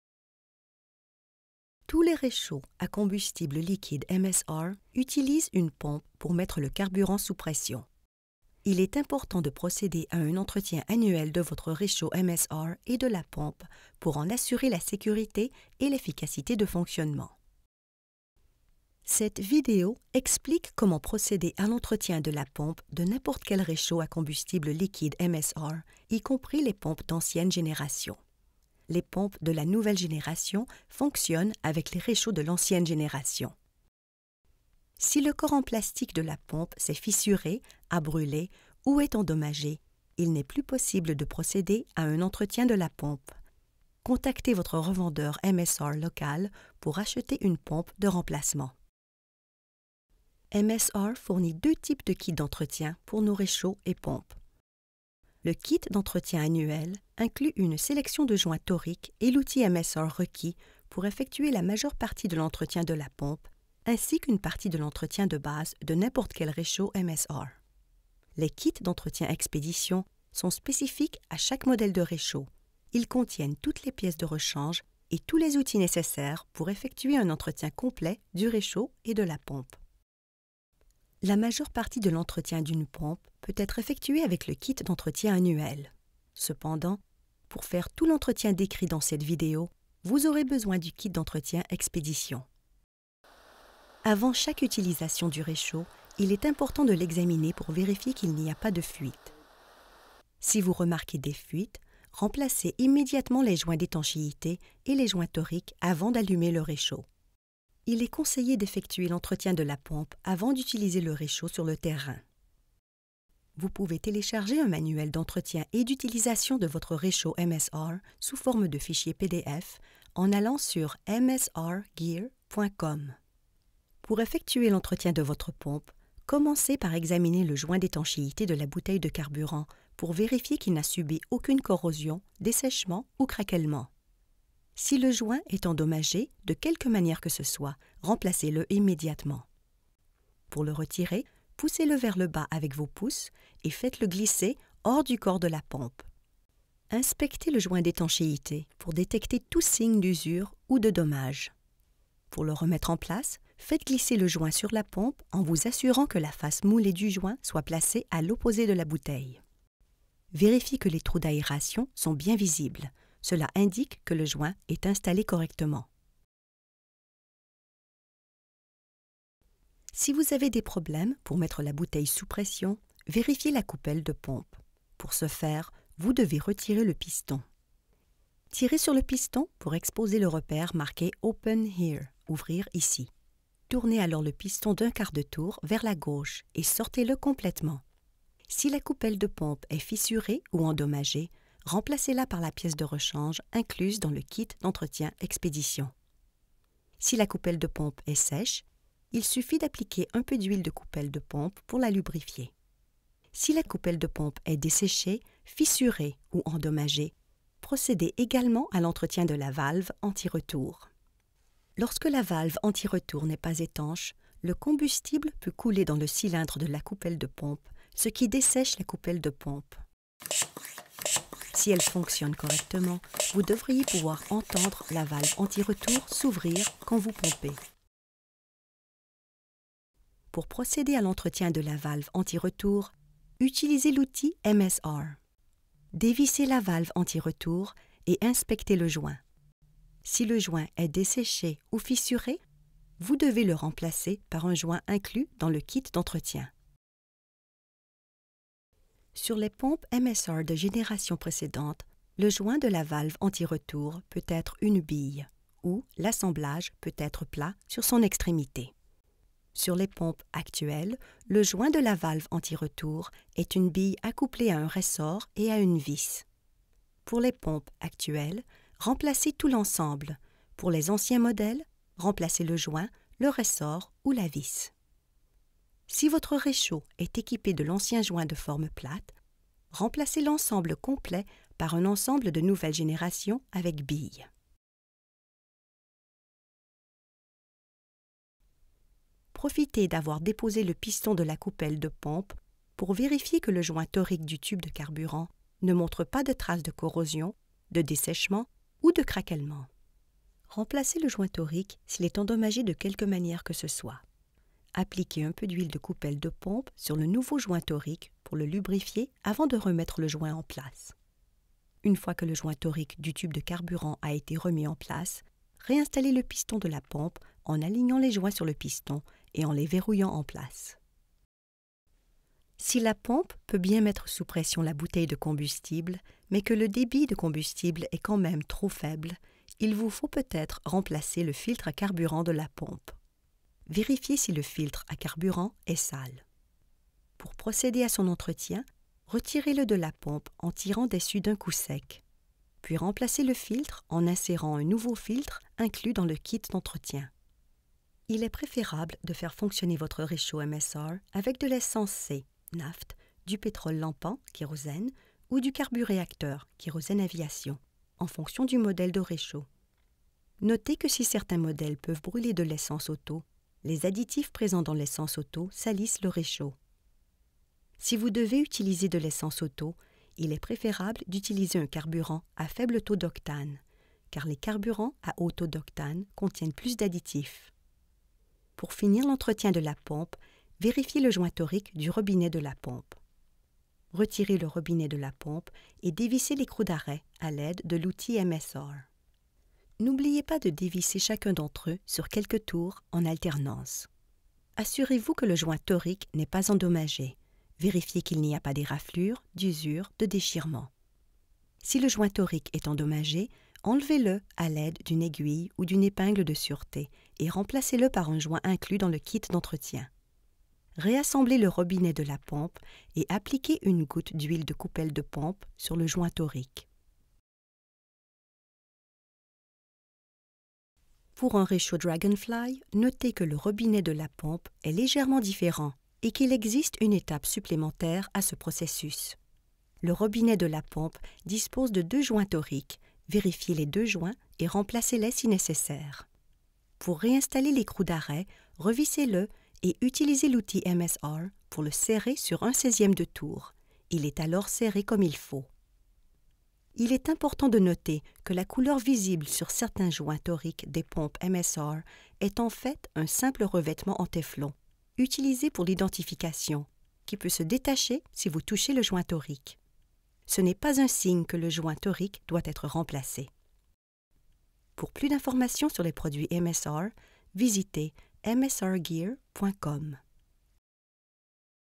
MSR Stoves: Entretien de la pompe du réchaud MSR (Francais)
Avec un entretien régulier, la pompe de votre réchaud à combustible liquide va durer pendant de longues années. Cette vidéo décrit les procédures d'entretien de base pour toutes les pompes de réchaud à combustible liquide MSR, y compris celles utilisées avec le réchaud Dragonfly, quelle que soit l'année de fabrication. Nous y abordons le dépannage et les réparations pour des problèmes tels que des fuites de carburant, la pression trop basse ou le débit de carburant trop réduit. Pour plus d'informations sur l'entretien et l'utilisation, rendez-vous sur msrgear.com pour télécharger le manuel complet de votre réchaud.